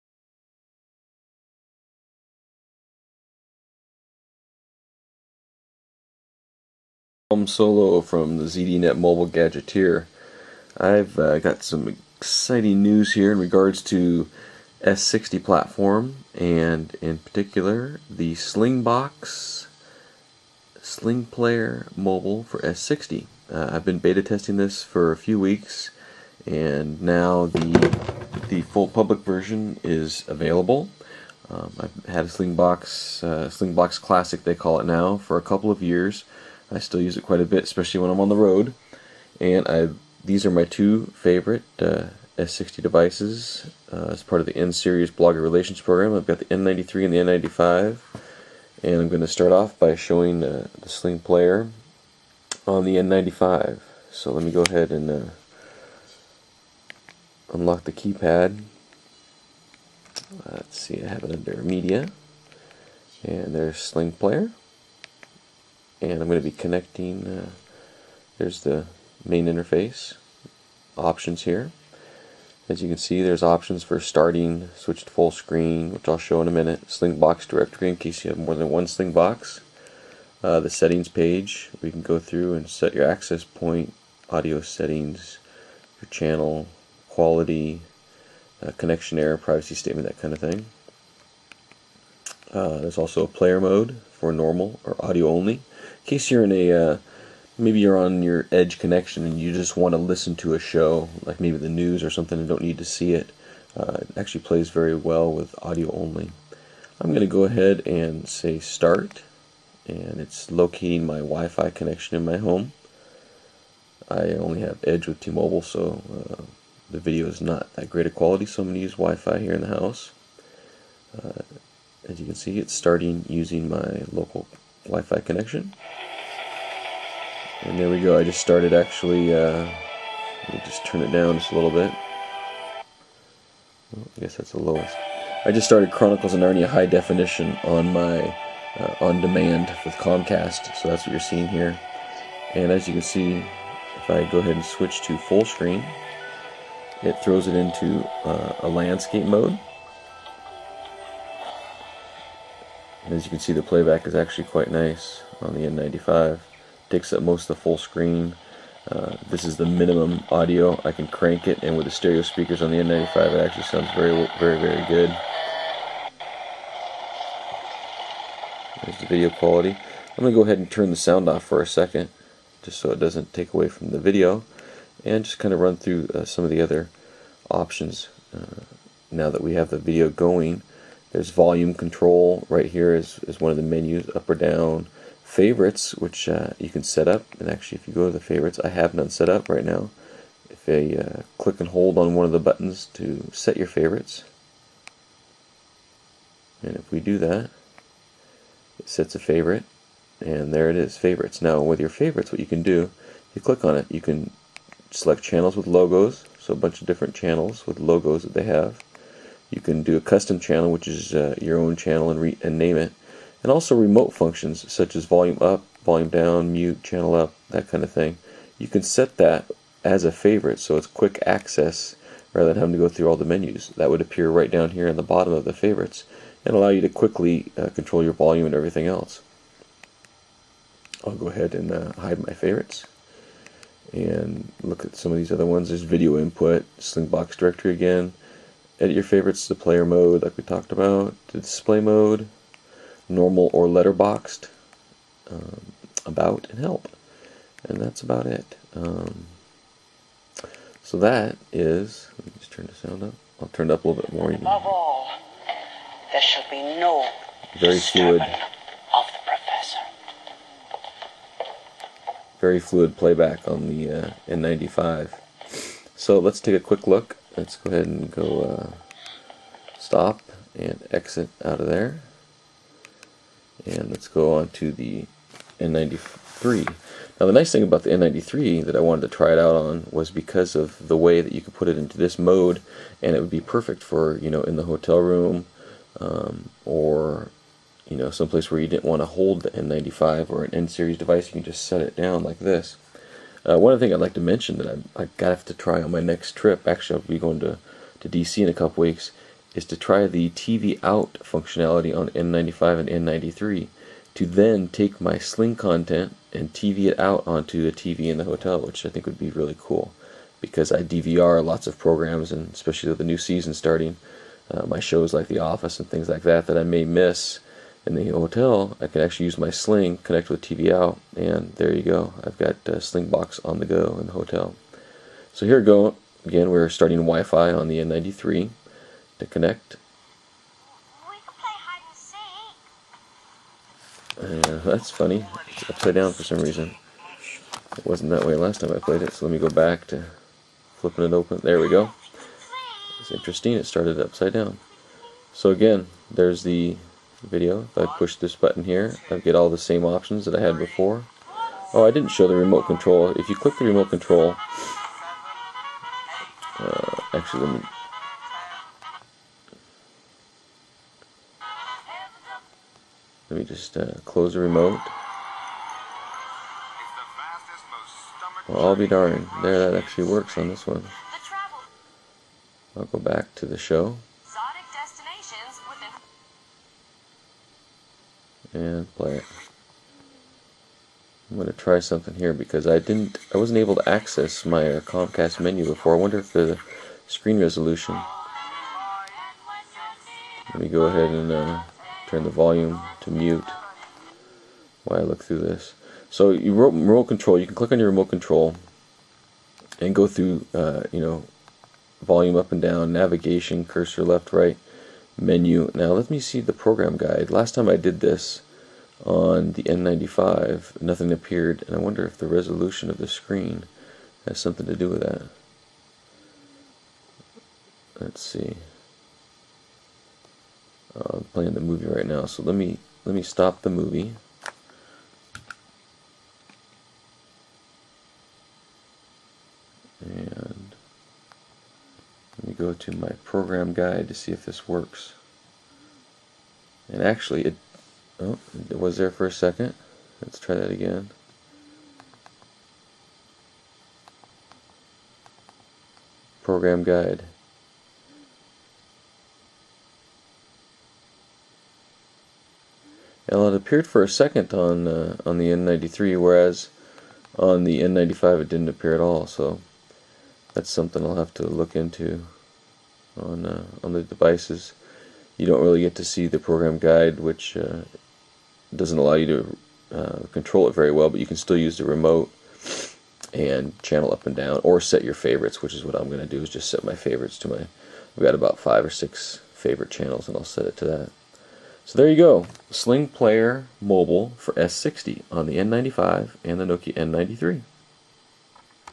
SlingPlayer Mobile for S60
There are mobile clients for Windows Mobile and Palm devices that allow you to view your Slingbox video content. Sling Media just announced the availability of their global S60 client application that works with the wildly popular Nokia N95.